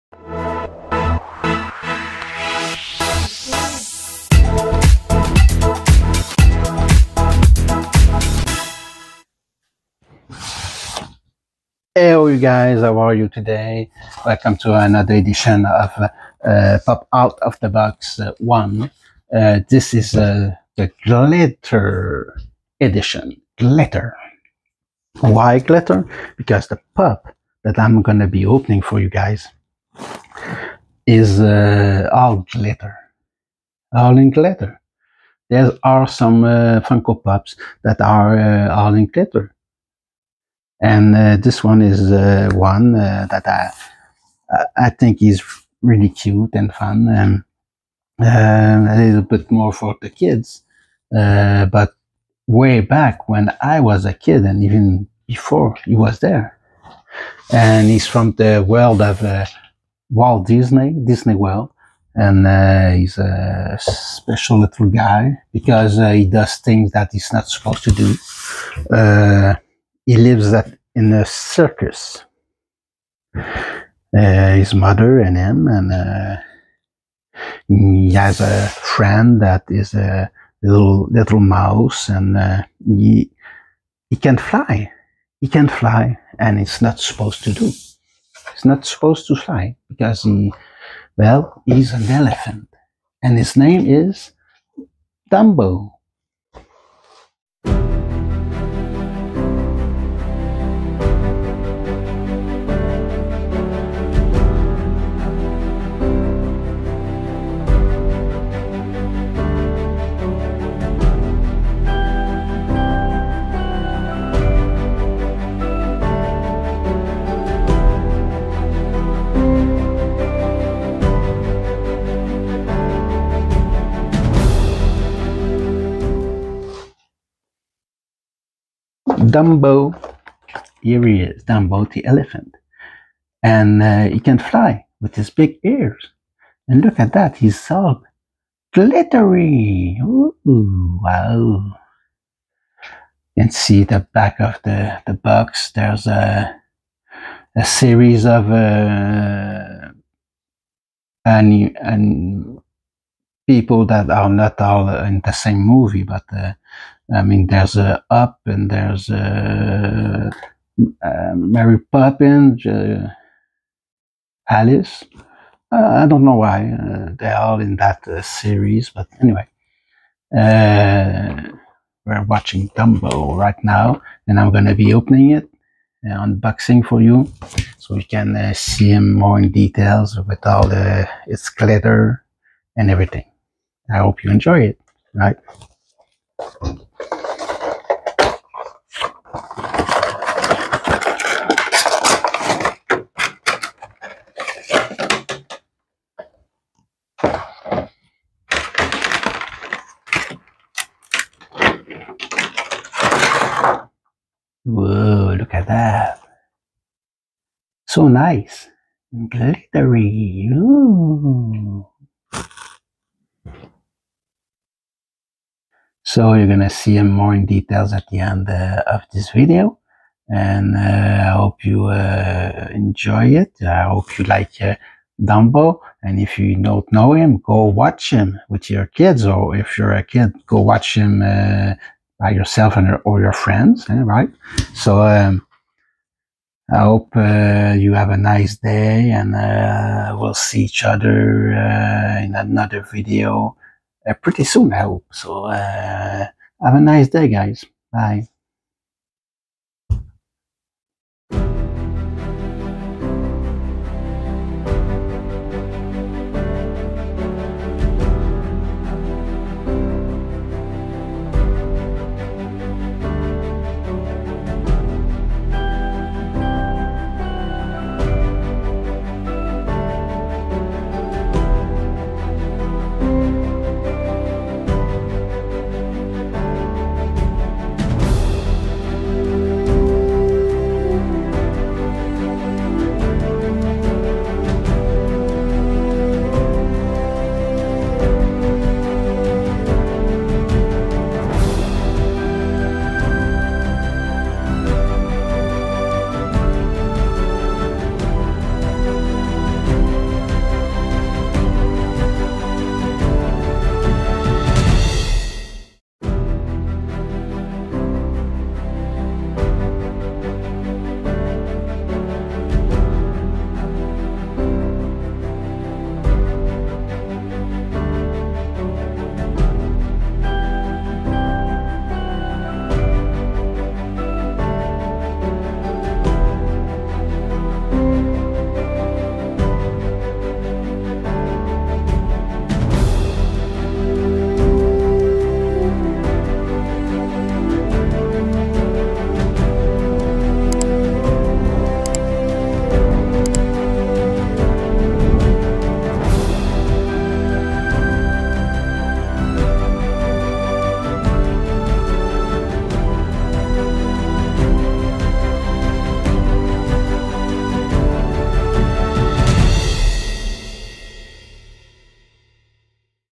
Unboxing a glitter funko pop from the world of Disney, Dumbo !
A glitter edition of pop out of box 1, from Disneyland , Dumbo the elephant !